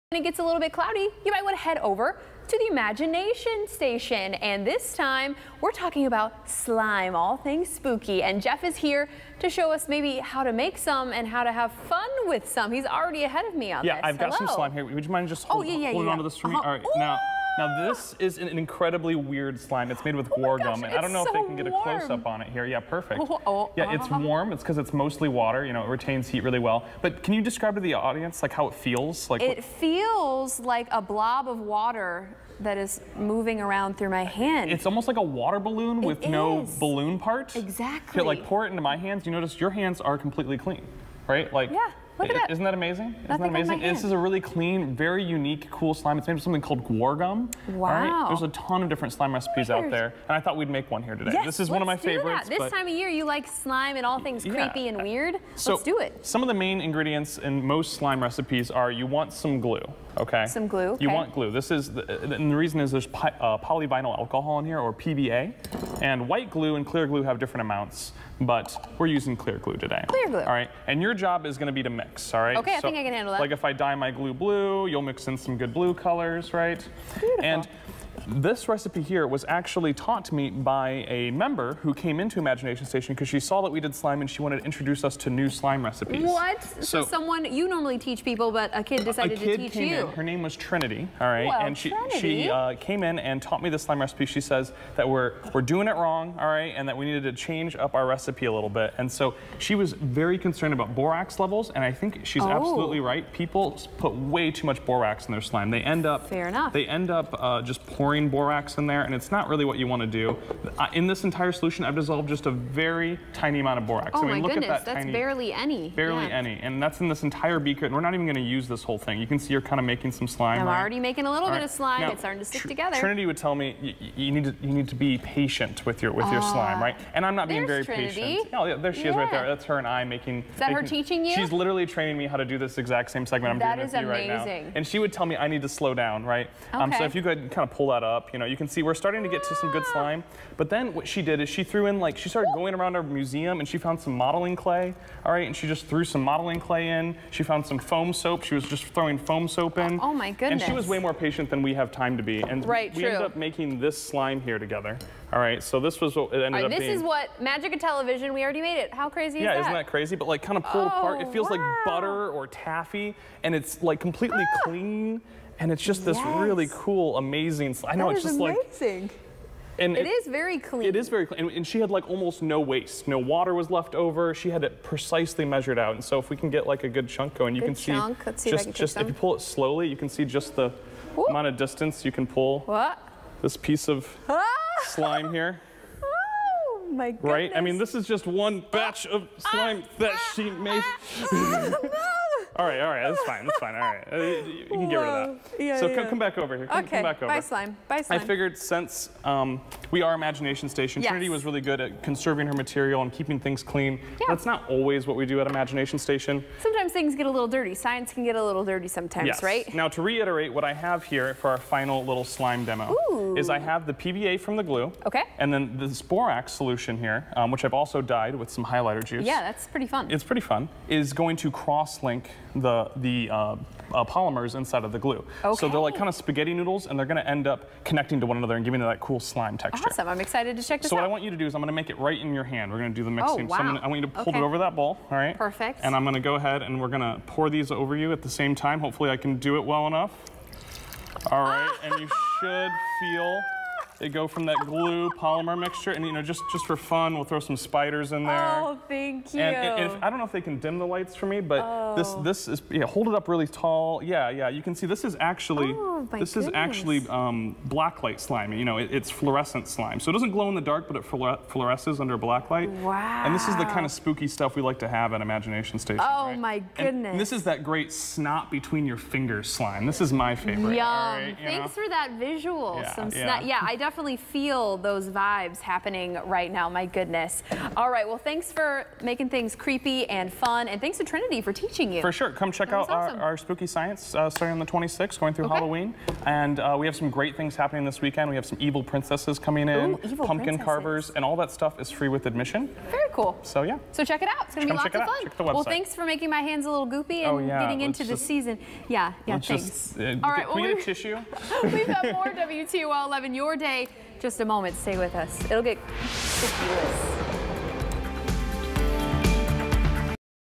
Making Slime
Extreme Scientist Jeff Stevenson whips up a batch of slime while talking explaining the science of polymers.